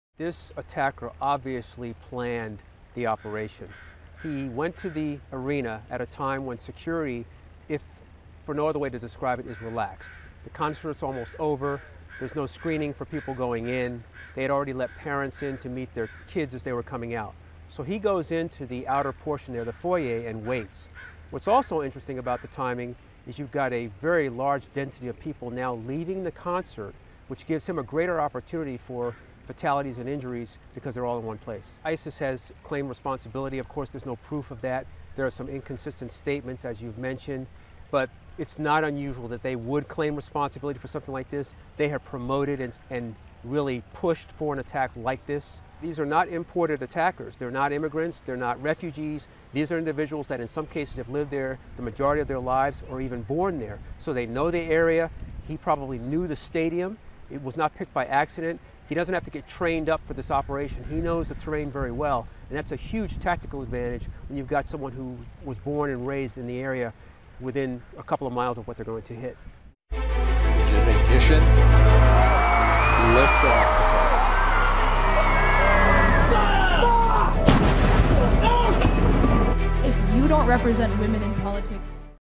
U.S. Security Expert Says Homegrown Terrorists Are A 'Huge Tactical Advantage' For ISIS
May 24, 2017 - Security and counter terrorism expert Erroll Southers says homegrown terrorists like Salman Abedi are a 'huge tactical advantage' for ISIS. Report by April Brown.